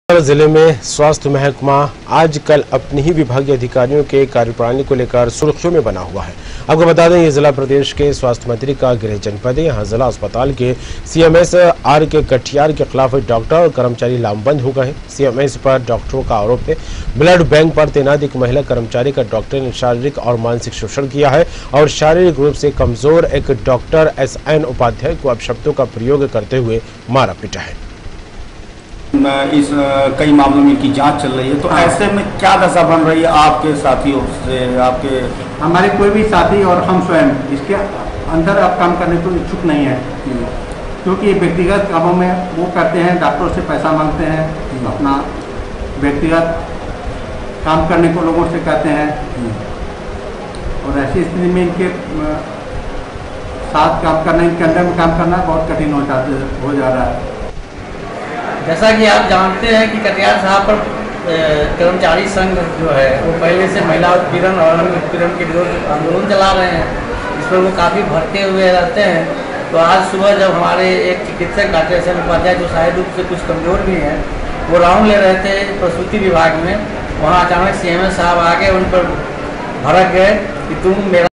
स्वास्थ्य विभाग के डॉक्टर पर आरोप Siddharthnagar | Uttar Pradesh
सिद्धार्थनगर जिले में स्वास्थ्य महकमा आज कल अपने ही विभागीय अधिकारियों के कार्य प्रणाली को लेकर सुर्खियों में है...आपको बता दे... यह जिला प्रदेश के स्वास्थ्य मंत्री का गृह जनपद है..जहां जिला अस्पताल के सीएमएस आर के कटियार के खिलाफ डॉक्टर और कर्मचारी लामबंद हो गये हैं ...सीएमएस पर डॉक्टरों का आरोप है... ब्लड बैंक पर तैनात एक महिला कर्मचारी का डॉक्टर ने शारीरिक और मानसिक शोषण किया है... और शारीरिक रूप से कमजोर एक डॉक्टर एस एन उपाध्याय को अपशब्दों का प्रयोग करते हुए मारा पीटा है